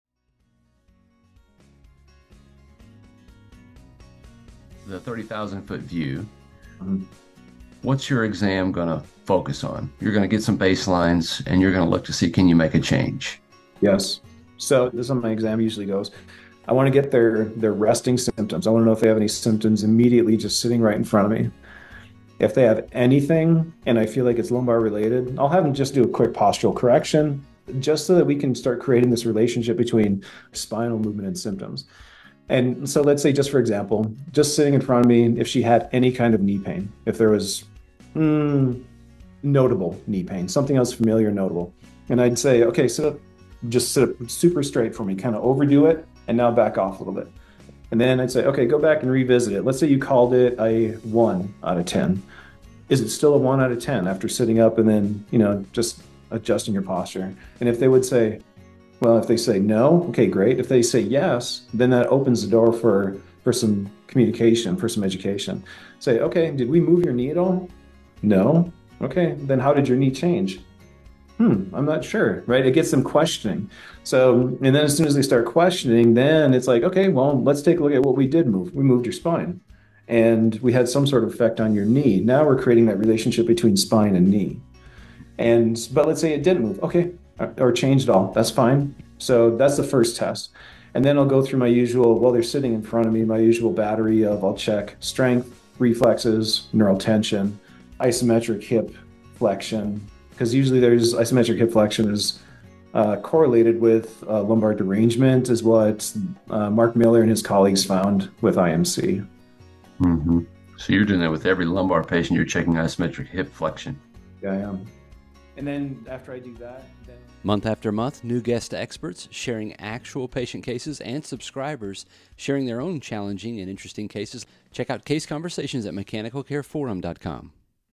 Initiating the Physical Exam
Are methodical and systematic with your physical exam?

Hear how Ryan is with each patient.

In Session 012401 we're joined by Mr. Ryan Tauzell of Wytheville, Virginia USA. Ryan shares a 69 year old female with persisting knee pain after 6 months and prior orthopedic care including conventional physical therapy and arthroscopy with no resolution. Ryan shares the patient's history, her response to physical exam testing, how he decided to pursue testing the spine versus the peripheral joint, the modifications needed to find full relief and recovery of her previous level of function. More discussion on spine screening, ruling out subgroups within the MDT classification of OTHER as well as some conversation on outcome tool measurements. This is a great case for those working in a setting where post surgical patients come some time after surgery still hurting.